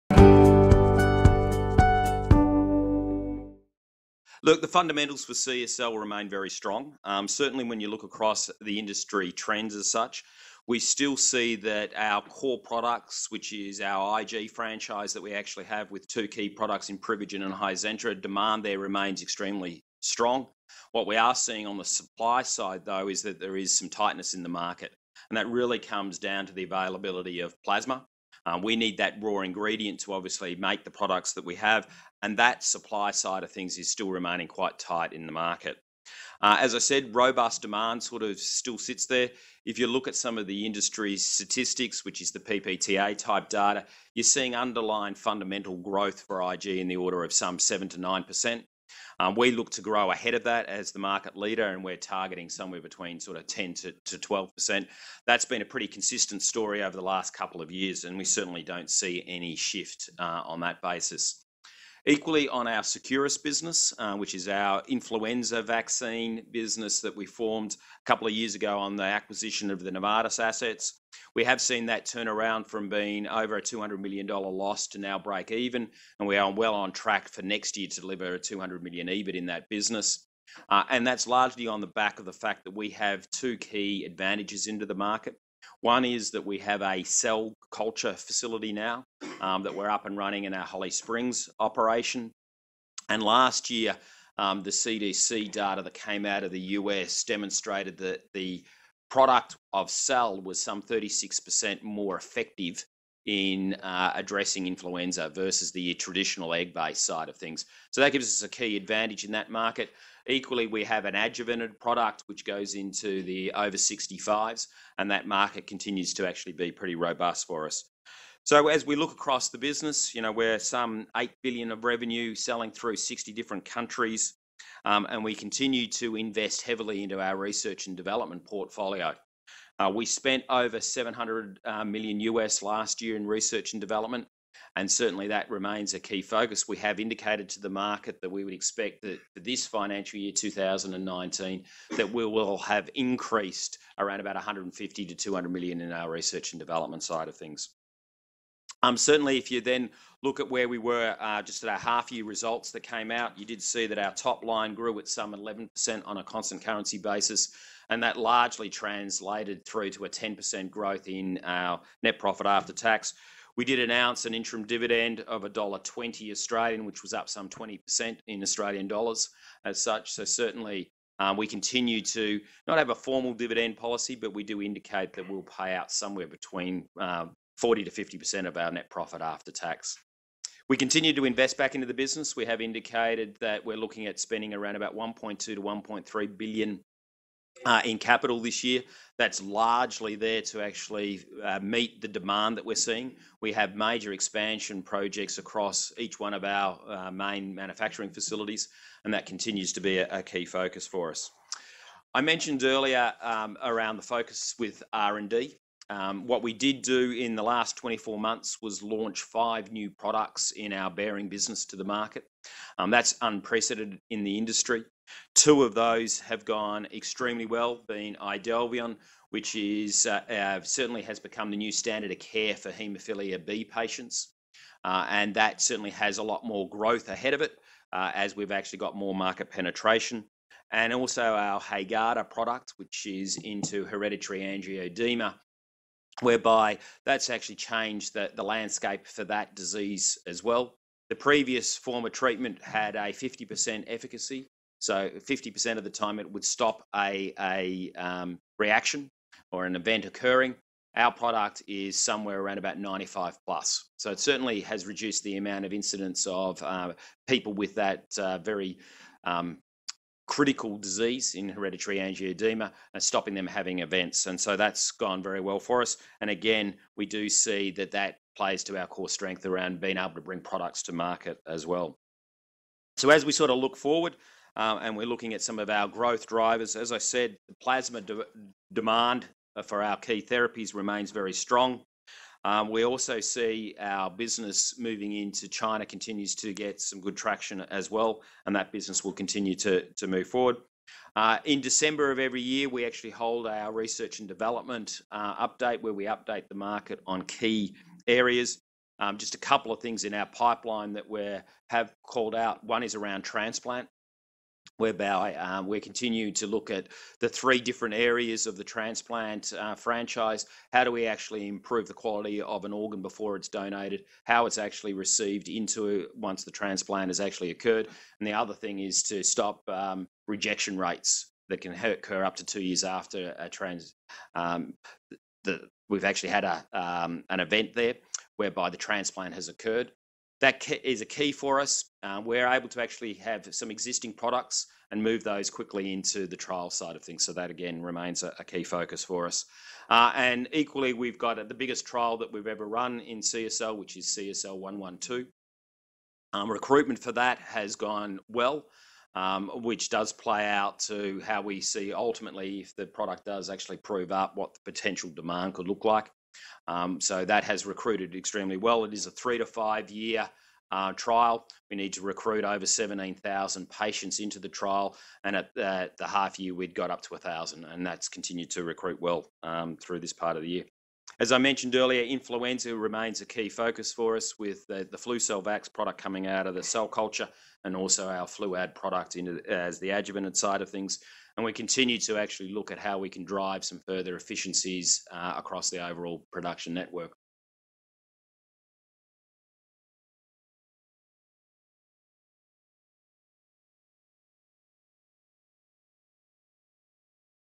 CSL Limited (ASX:CSL): David Lamont, Chief Financial Officer
CSL Limited (ASX:CSL) Chief Financial Officer David Lamont provides an update on the company to the Morgans network.